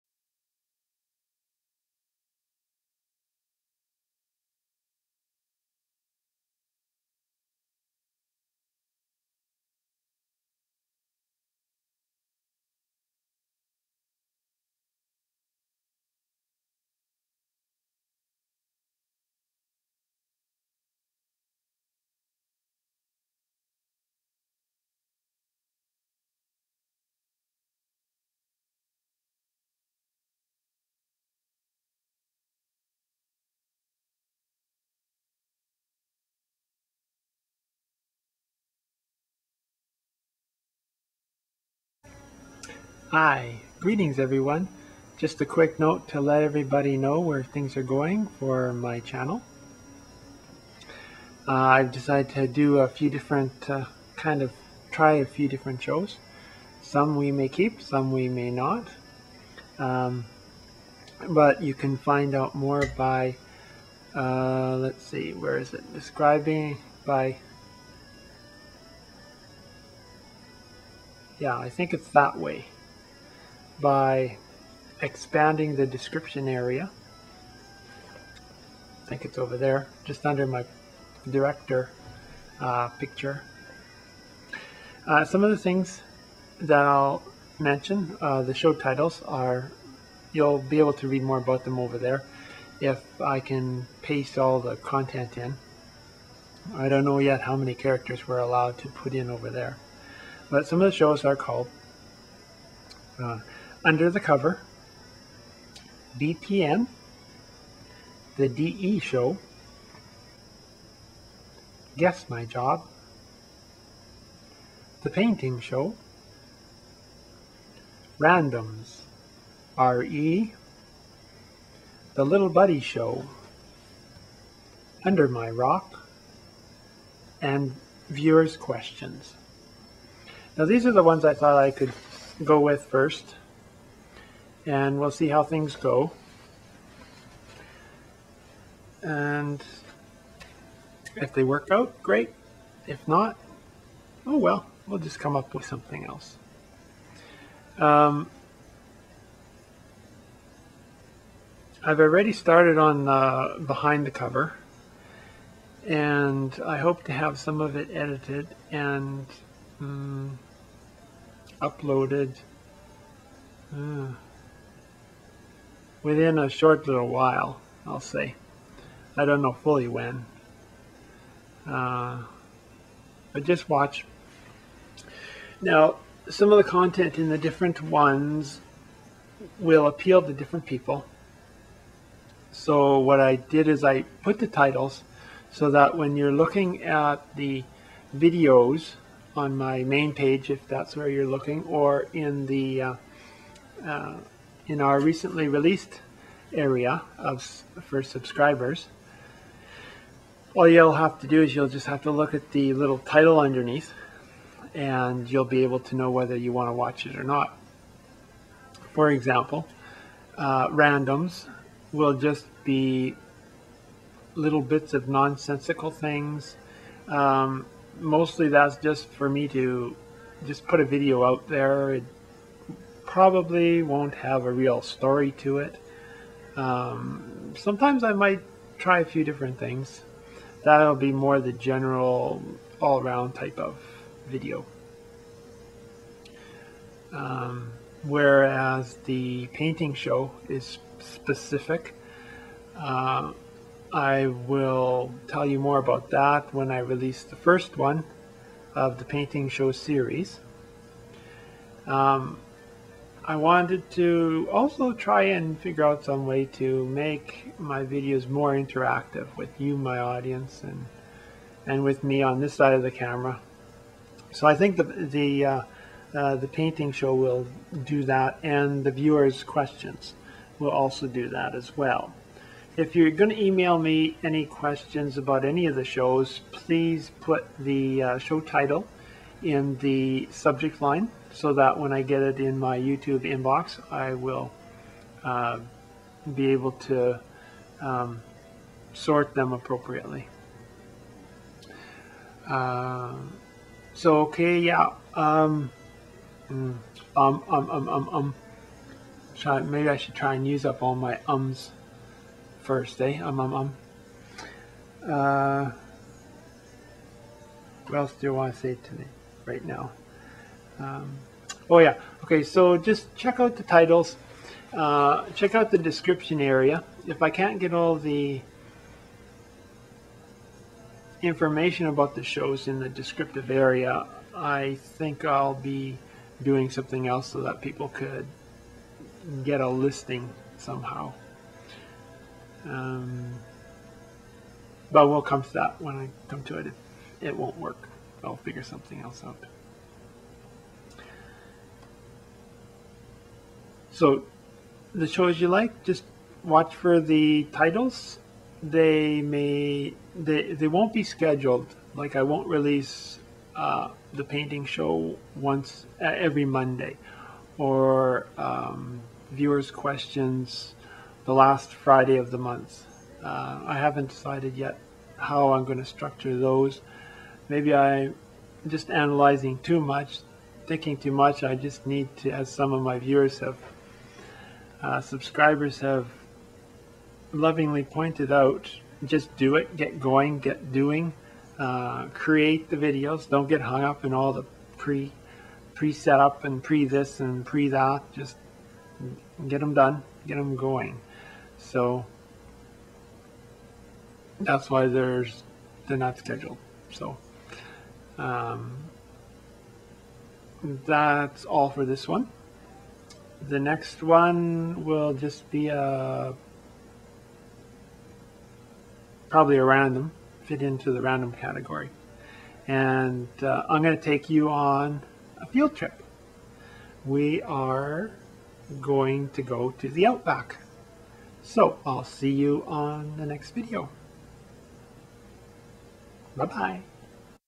BryansPlace TV Network
A Message from the management of BP TV. (Please note the shows and show titles on the BP TV network are still in negotiations and are not yet signed contracts. Different ones may be added and others may be dropped at the whim of management. We (The Management) reserve the right to air the shows we want and when we want so no specific broadcast days or order have been set. If you want to send an email on a specific show please reference the show in the subject line of your email). Thank you and now for our current lineup...

Behind The Cover - A serious multi-part look at the life and times of Published Author Bryan Hood. Delve into his past to find out what it took to be what he is today.

BPN - BryansPlace News (BPN) covers news and happenings (tongue in cheek at times) in and around the YouTube community and other places. A light hearted look with (at times) a dose of satire. (BP TV Network Management Note: If a reference to you or some of your videos have been used on BPN and you no longer wish to be the receiver of such attention - private message the management and we will ensure that you and or your content is not used in and or with any future creations, thank you).

The D.E. Show - Dimensional Existence (D.E.) explores the potential and possibilities of Human-Beings. Featured host and published author Bryan Hood will share his thoughts, ideas and findings on the subject.

Guess My Job - The Guess My Job Show is a limited show run. It will end with the completion being when someone actually correctly guesses my job, (with the correct terminology). There will be an actual prize (yet to be determined) for the winner. You will receive clues that will relate in someway to my job. There might be an object in the video that relates to my job. The overall video it's self may be the clue.

The Painting Show - This show will take you on a creative journey from start to completion of an actual acrylic painting. The style used by the artist has been called - folkartsy with a touch of innocent beginnerism.

Randoms -- Randoms are a collection of secret videos (some funny, some plane stupid) that have been painstakingly snuck out of the BP TV Network headquarters under management's nose. These were never meant to be seen ever (publically or privately). If our undercover operatives are caught, he/she will be terminated on the spot. And we are not talking pink slips here, so enjoy these.

RE: - RE:'s will be reply videos to our other YouTubeians.

The Little Buddy Show. - Life and times from the perspective of a humble pet who thinks he is a person.

In The Loop. - Questions and pondering from the outer rim.

Viewers Questions??? -- Bryan will answer and address the submitted viewer's questions. Note: If you send a question by private message (YouTube email) to "Viewers Questions" and you do not want it answered on camera please say so (politely) in that email and it will be kept private. To submit a question to Bryan please send an email with the subject line: "Viewers Questions".

Management of the BP TV Network reserve the right to change and or modify the above without any prior written or implied notices of and or in any form to you OR your pet at any time, any place, for any resin, yadda, yadda, yadda. You know the rest.